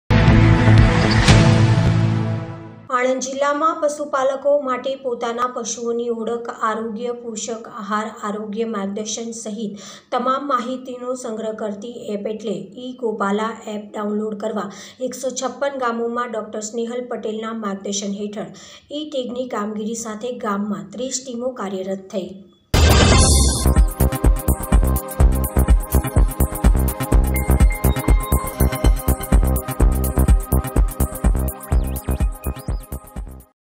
આણંદ જિલ્લામાં પશુપાલકો માટે પોતાના પશુઓની ઓળખ ,આરોગ્ય ,પોષક,આહાર , આરોગ્ય માર્ગદર્શન કામગરી ....
આણંદ જિલ્લામાં પશુપાલકો માટે પોતાના પશુઓની ઓળખ ,આરોગ્ય ,પોષક,આહાર , આરોગ્ય માર્ગદર્શન સહીત તમામ માહિતીનો સંગ્રહ કરતી એપ એટલે ઈ-ગોપાલા એપ ડાઉનલોડ કરવા ૧૫૬ ગામોમાં ડો.સ્નેહલ પટેલના માર્ગદર્શન હેઠળ  ઈ-ટેગની કામગરી સાથે ગામે ગામ  ૩૦ ટીમો કાર્યરત થઈ.